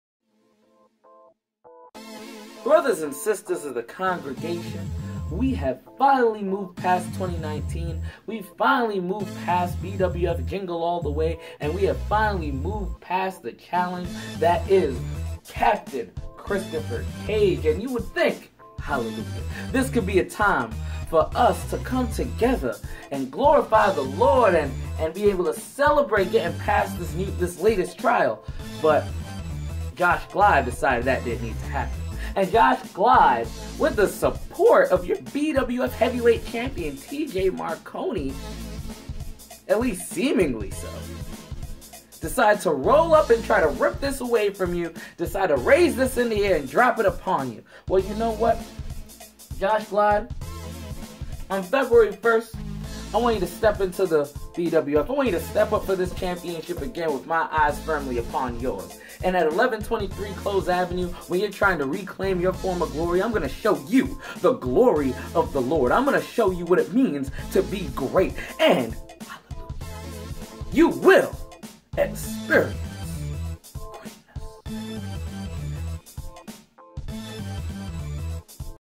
Brother Greatness' message to Josh Glide
Josh Glide attacked at bwf jungle all the way dec 2019. At a bronx tale on Feb 1st 2020 retribution will be had.